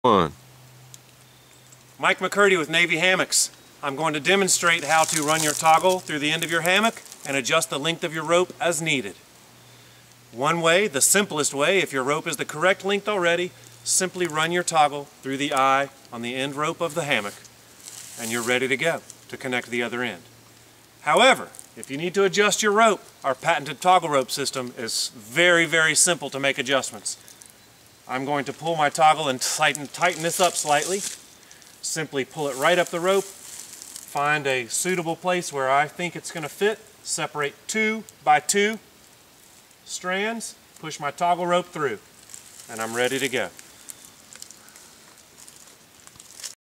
Toggle Rope Demonstration
Mike McCurdy of Navy Hammocks (NAVYHAMMOCKS.COM) demonstrates one of many ways to use our patented Toggle ropes. Navy Hammocks. The ultimate ride. Sailor Made in the USA